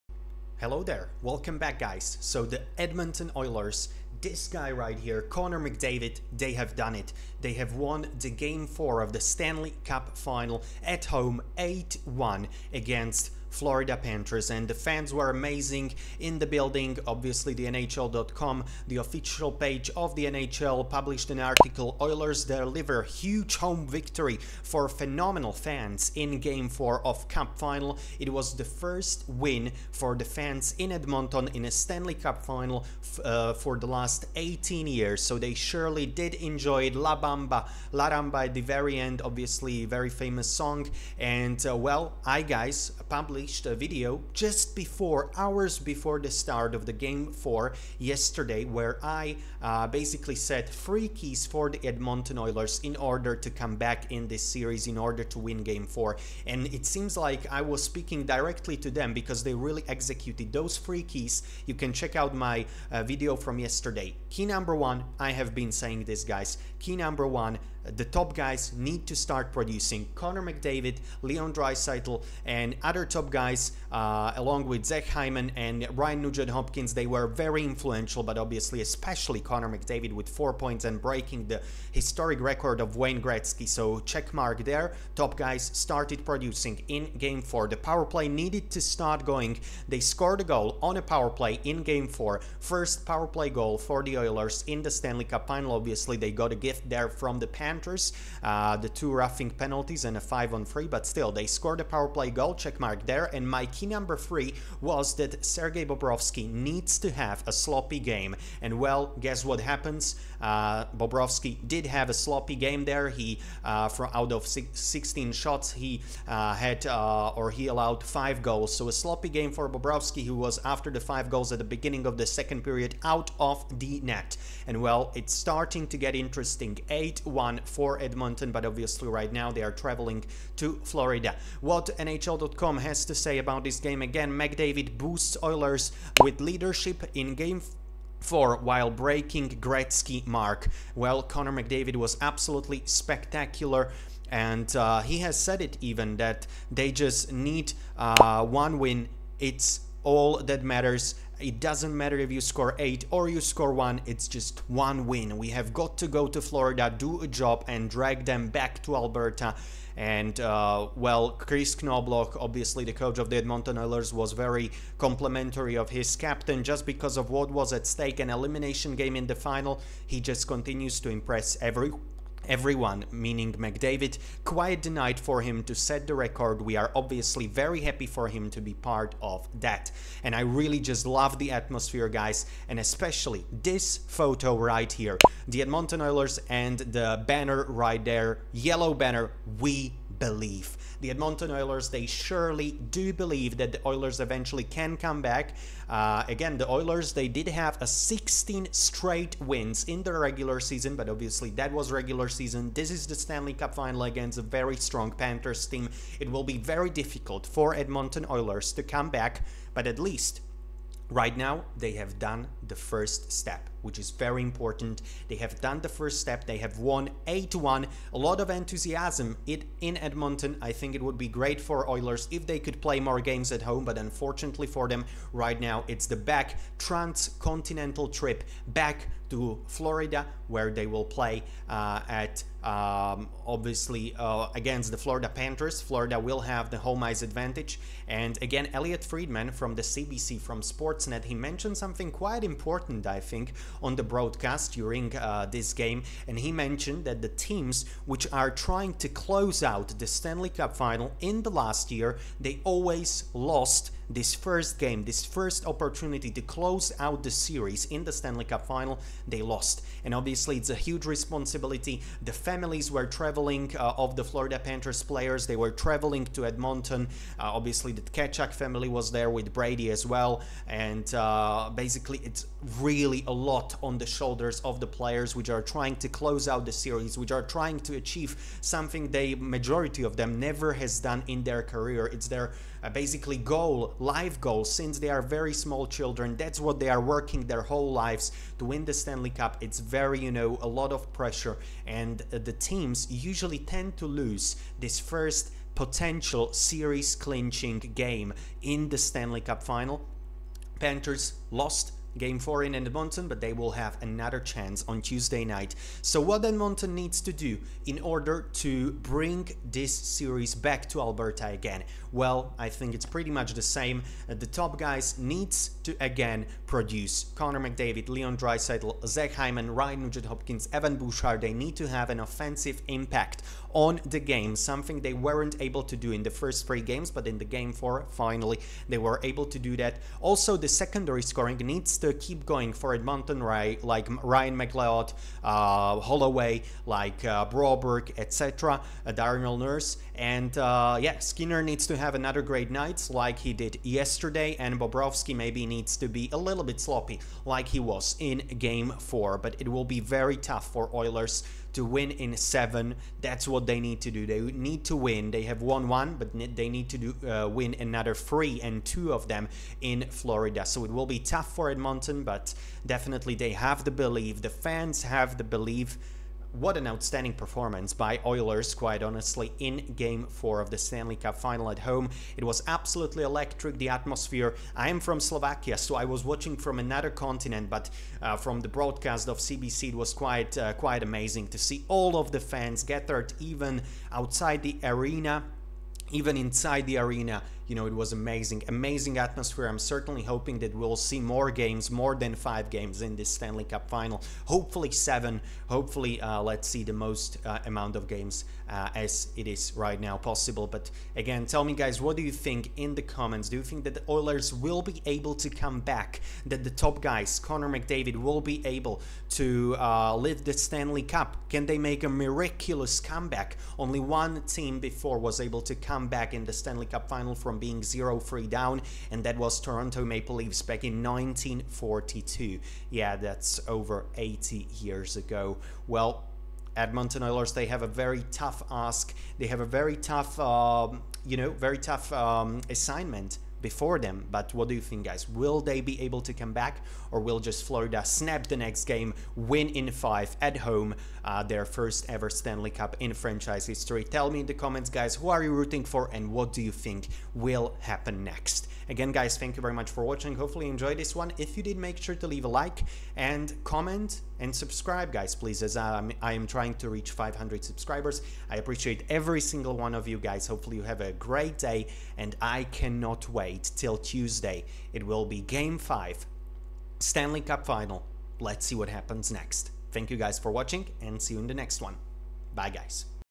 Oilers Are BACK IN IT!
Hi guys, in today's video we will be talking about Edmonton's big win in Game 4 of Stanley Cup Final. What are the keys for them in order to succeed?

What do you think, can Oilers turn this series around?